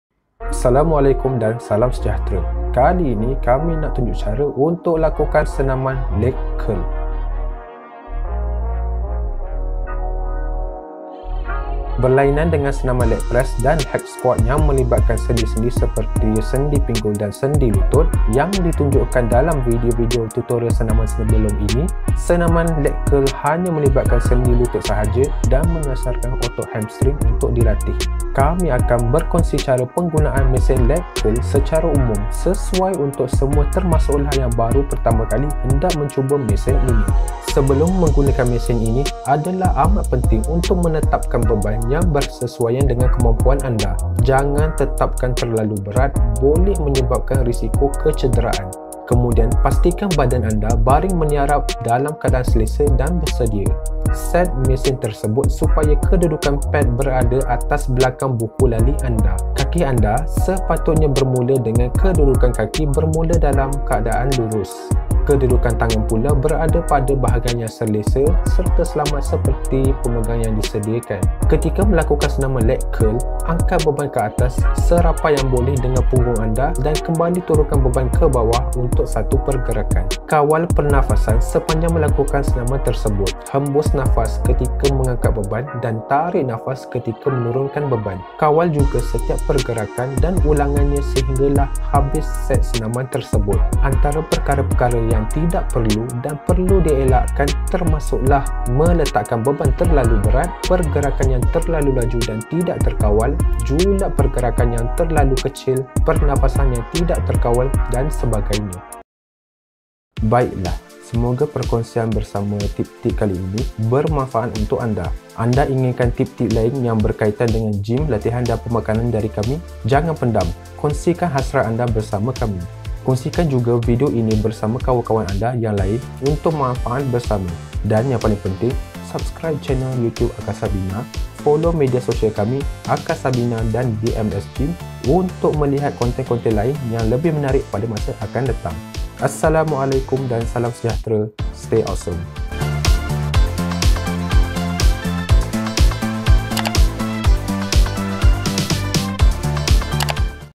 Tutorial Workout Senaman Kaki LEG CURL [Akasa Bina X DMS Gym]
Assalamualaikum dan salam sejahtera dari AB.

Hasil kerjasama antara Akasa Bina dan DMS Gym, video tutorial senaman workout akan dikongsikan bersama dalam channel ini untuk tujuan penerangan senaman bagi memastikan semua yang menonton dapat mempelajari teknik senaman tersebut dengan berkesan.

Kali ini, kami akan berkongsi tutorial workout cara senaman kaki iaitu senaman LEG CURL. Ianya antara variasi senaman kaki yang berkesan dan efektif untuk membantu besarkan dan kuatkan bahagian belakang peha iaitu kumpulan otot hamstring.

Selamat menonton dan semoga mendapat sedikit sebanyak manfaat darinya.